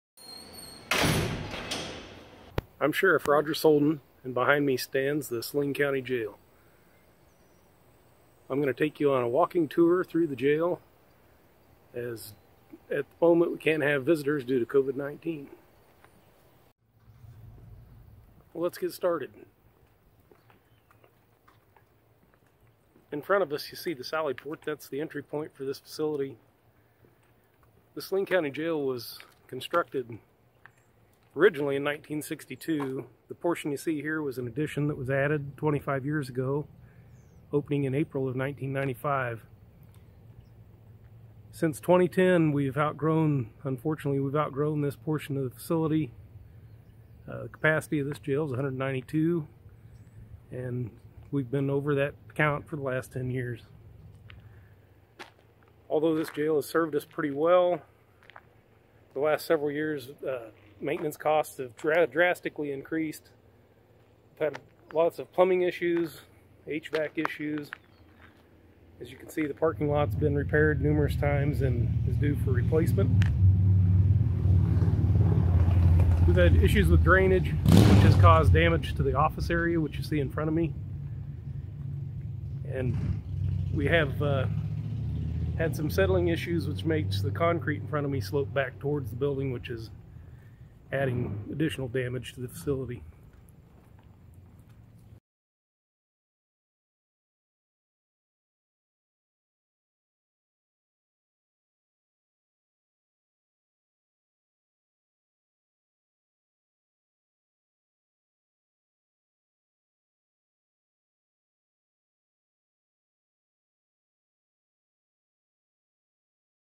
Saturdays in the Slammer - Week 1 Tour of Outside the Saline County Jail
Due to COVID-19, we are unable to conduct tours of the facility.  Join Sheriff Roger Soldan on a virtual tour of the Saline County Jail.